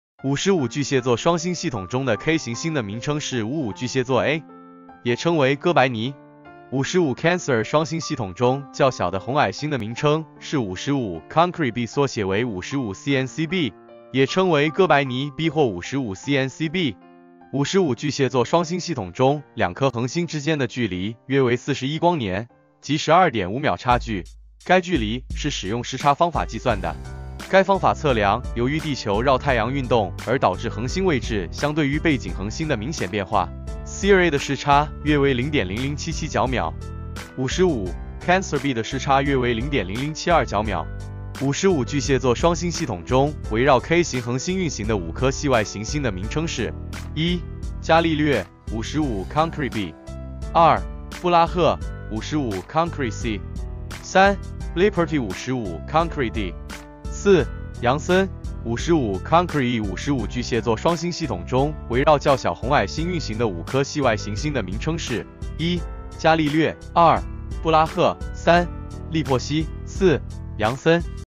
55 巨蟹座：擁有 5 顆系外行星的神秘雙星系統
55巨蟹座雙星系統中的K型星的名稱是55巨蟹座A，也稱為哥白尼。55 Cancer雙星系統中較小的紅矮星的名稱是55 Cancri B（縮寫為55 Cnc B），也稱為哥白尼B或55 Cnc B。55巨蟹座雙星系統中兩顆恆星之間的距離約為41光年，即12.5秒差距。該距離是使用視差方法計算的，該方法測量由於地球繞太陽運動而導致恆星位置相對於背景恆星的明顯變化。cer A 的視差約為 0.0077 角秒，55 Cancer B 的視差約為 0.0072 角秒。55巨蟹座雙星系統中圍繞K型恆星運行的五顆系外行星的名稱是：

1. 伽利略 (55 Cancri b)
2. 布拉赫 (55 Cancri c)
3. Lipperhey (55 Cancri d)
4.楊森（55 Cancri e）55 巨蟹座雙星系統中圍繞較小紅矮星運行的五顆系外行星的名稱是：

1.伽利略
2.布拉赫
3.利珀希
4. 楊森